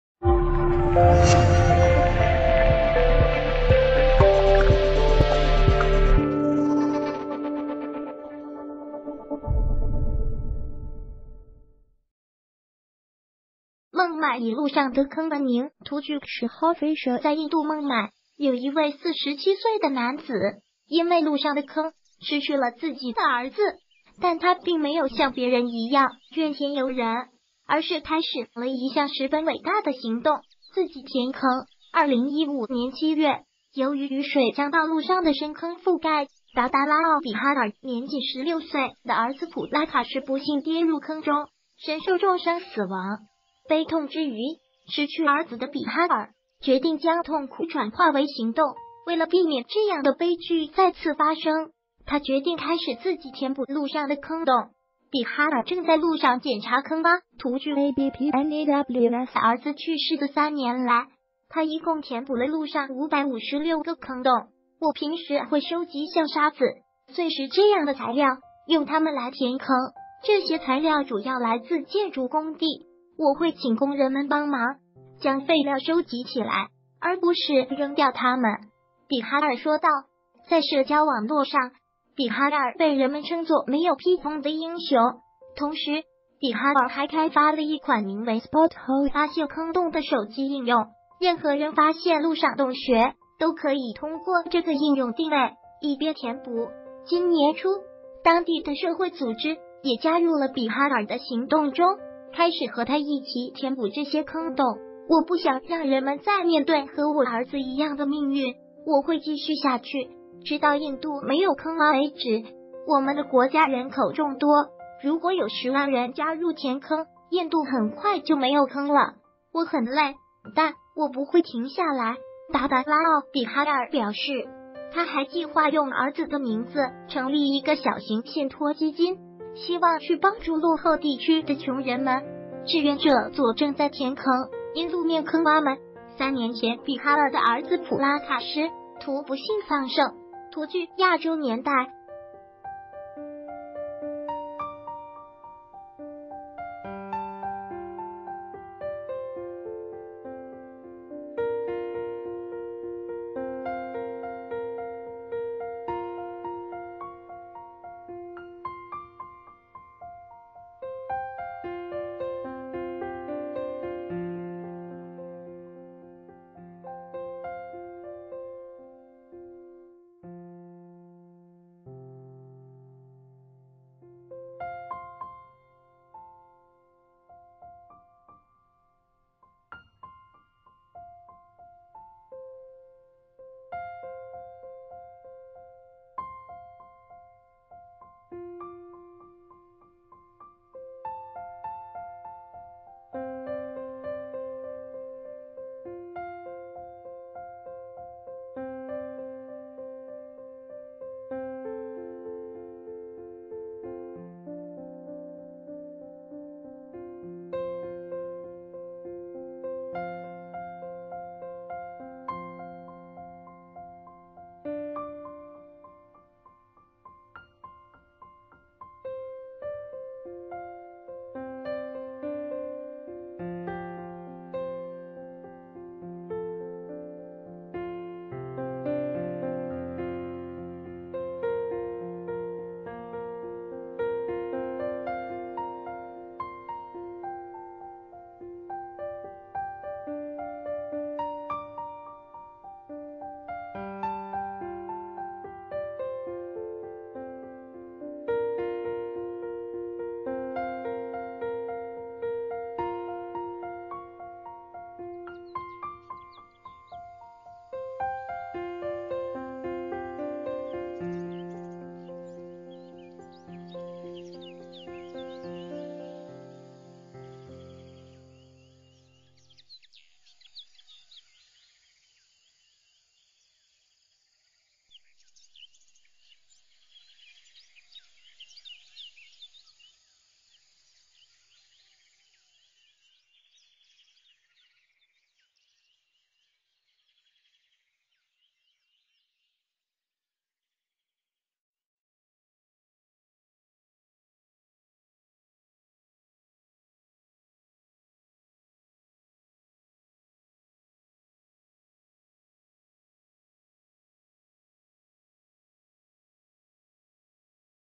儿子跌入坑中丧命 父亲在"最坑城市"填556个坑
谢谢！孟买以路上的坑闻名图据kshahofficial在印度孟买，有一位47岁的男子，因为路上的坑，失去了自己的儿子。但他并没有像别人一样怨天尤人，而是开始了一项十分伟大的行动：自己填坑。2015年7月，由于雨水将道路上的深坑覆盖，达达拉奥·比哈尔年仅16岁的儿子普拉卡什不幸跌入坑中，身受重伤死亡。悲痛之余，失去儿子的比哈尔决定将痛苦转化为行动。为了避免这样的悲剧再次发生，他决定开始自己填补路上的坑洞。比哈尔正在路上检查坑洼图据ABPNEWS儿子去世的三年来，他一共填补了路上556个坑洞。“我平时会收集像沙子、碎石这样的材料，用它们来填坑。这些材料主要来自建筑工地，我会请工人们帮忙，将废料收集起来，而不是扔掉它们。”比哈尔说道。在社交网络上，比哈尔被人们称作“没有披风的英雄”。同时，比哈尔还开发了一款名为Spothole（发现坑洞）的手机应用，任何人发现路上洞穴，都可以通过这个应用定位，以便填补。今年初，当地的社会组织也加入了比哈尔的行动中，开始和他一起填补这些坑洞。“我不想让人们再面对和我儿子一样的命运。我会继续下去，直到印度没有坑洼为止。我们的国家人口众多，如果有10万人加入填坑，印度很快就没有坑了。我很累，但我不会停下来。”达达拉奥·比哈尔表示。他还计划用儿子的名字成立一个小型信托基金，希望去帮助落后地区的穷人们。志愿者（左）正在填坑；因路面坑洼，三年前比哈尔的儿子普拉卡什（右图）不幸丧生。图据《亚洲年代》